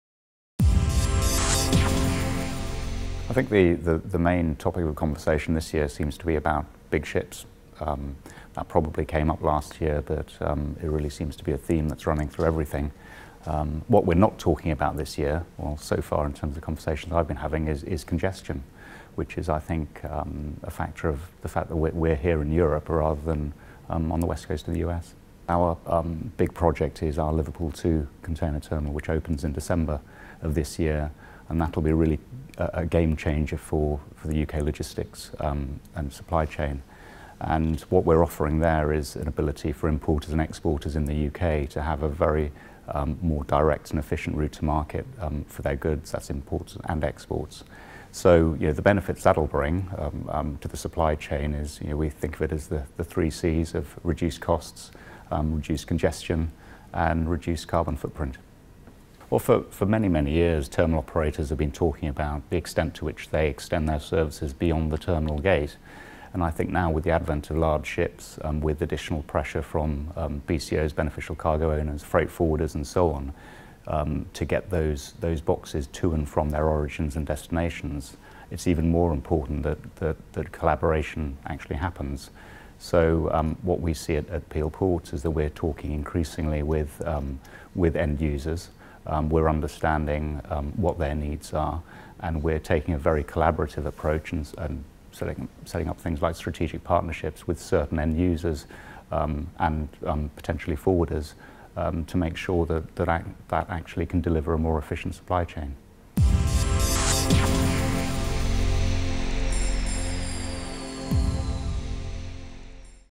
The Three Essential Goals of Liverpool2
PTI recently spoke with Patrick Walters, Commercial Director of Peel Ports, at TOC Europe 2015 in Rotterdam. Mr Walters highlights the ongoing Liverpool2 expansion project in the UK, which is designed to give forwarders a more direct and efficient route to market for their goods.